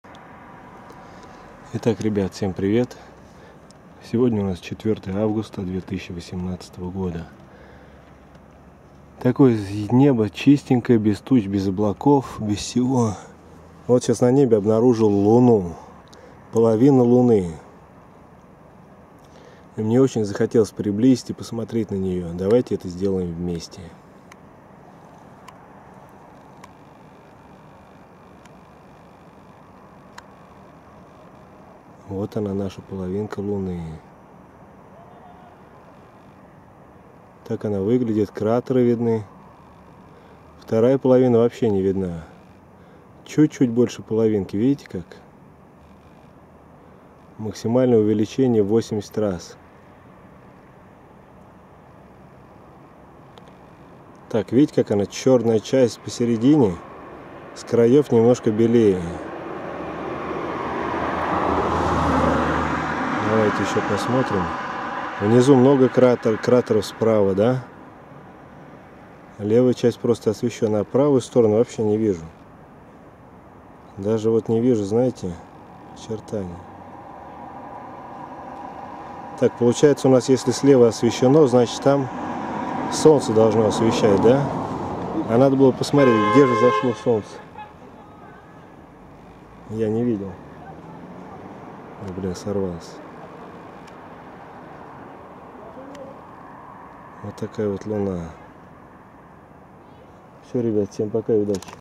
Половинка Луны! 80-ти кратное увеличение, кратеры и ТЁМНАЯ сторона (август 2018)!
На поддержку и развитие канала!